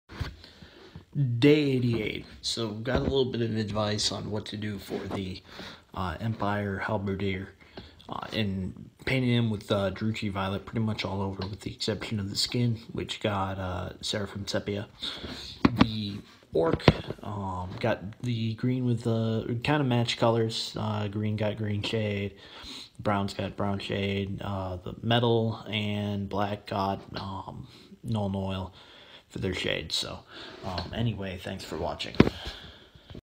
Day 88 - Orc and Human Shading
I shaded the Empire Halberdier and Orc Boy based on recommendations. They'll be dried, a new layer installed, and finished tomorrow.